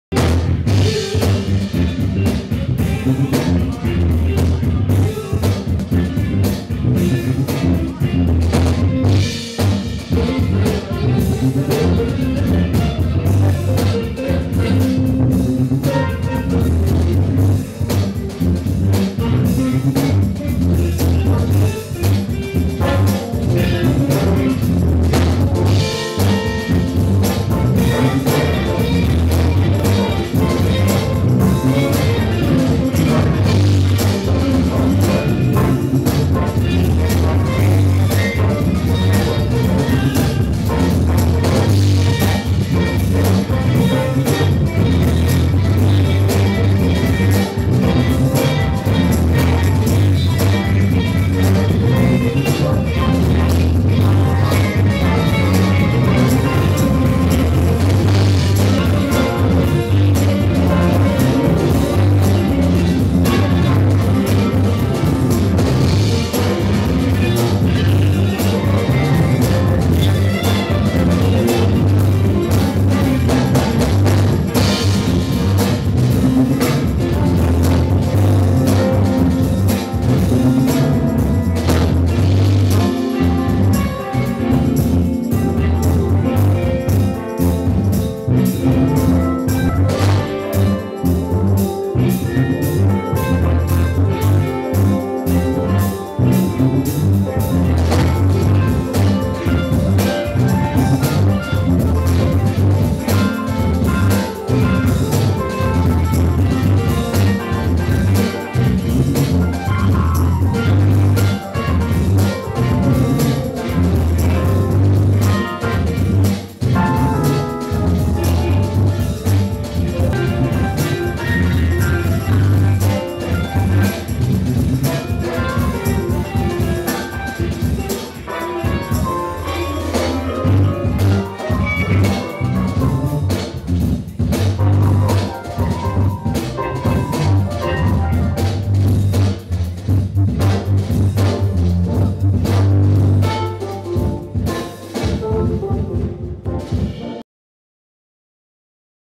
just a groove
playing tambourine(again) with james poyser (drums), owen biddle (bass), cap'n kirk douglas (guitar), and alan holzman (keys) during soundcheck before The Roots' jam session - 4/29/09 - NYC Highline Ballroom